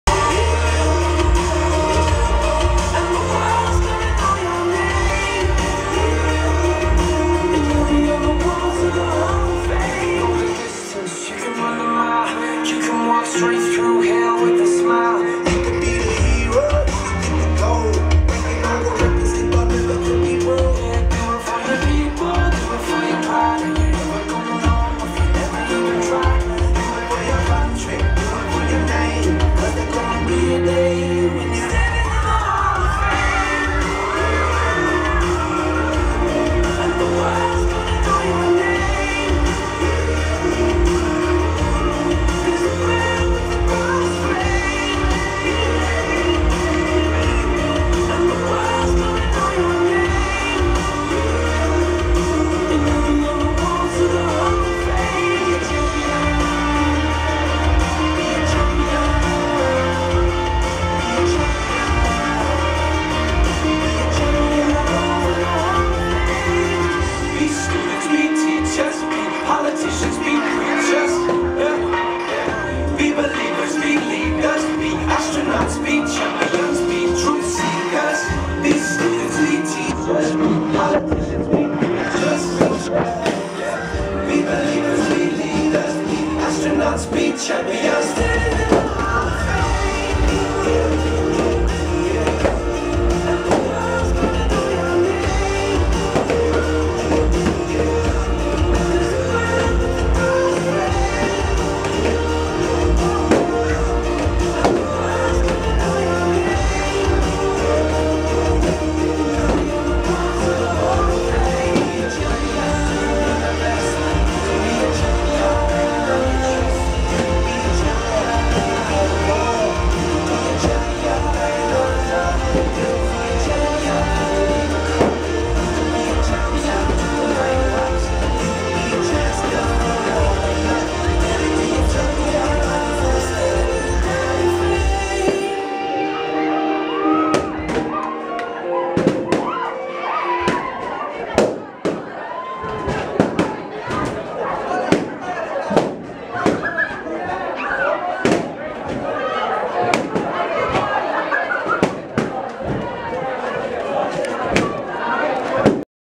Harrow Way Prom 2013 - Balloon Drop and Final Dance
Another Harrow Way leavers do - the balloon drop and final song.

Hope all who attended enjoyed the evening. :)